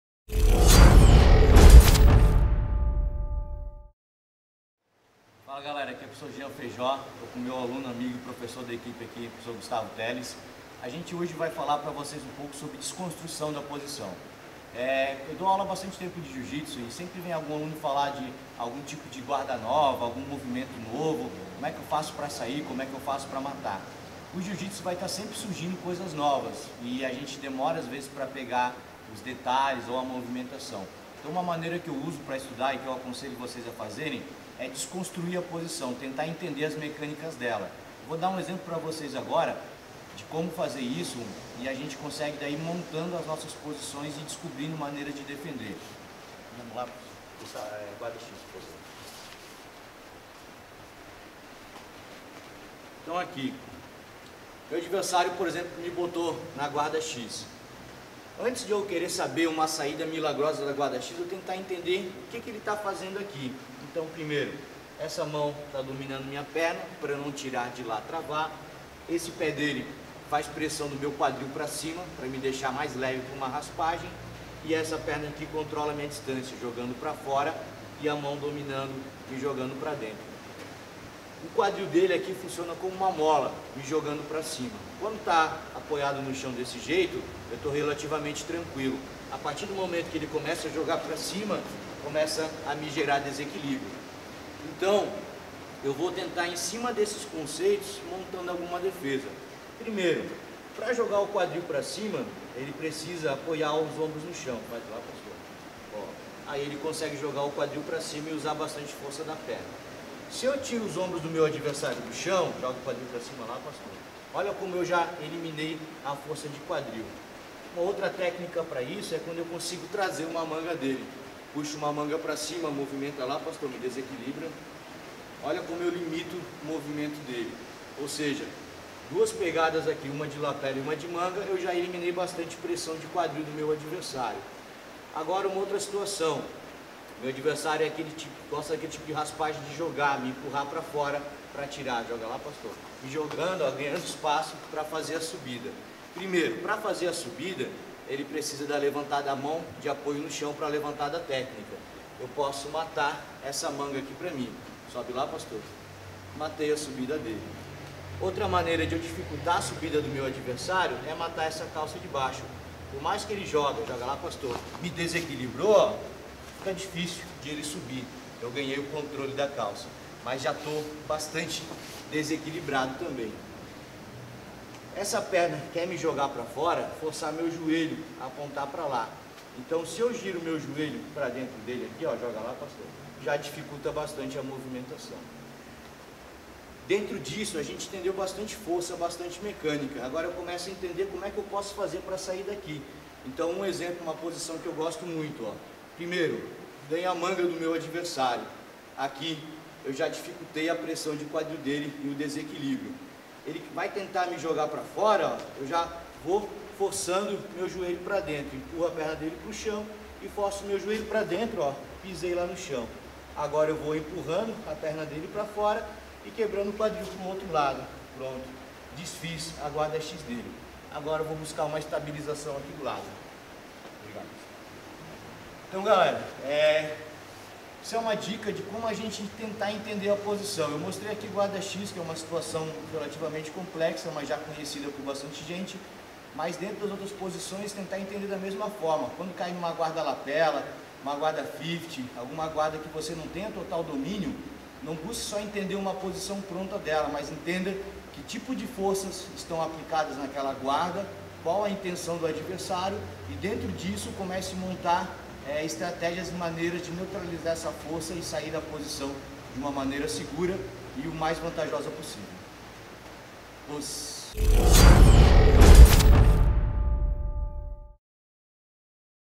Jean Feijó ensina a desconstruir posições e criar antídotos no Jiu-Jitsu
Como criar defesas para aquela guarda enjoada do seu parceiro de treinos, ou uma transição que sempre te pega nos campeonatos? Além da tradicional aula do professor faixa-preta, que sempre tem na manga uma solução, que tal experimentar uma desconstrução com estudo das técnicas? É o que sugere nosso professor GMI Jean Feijó.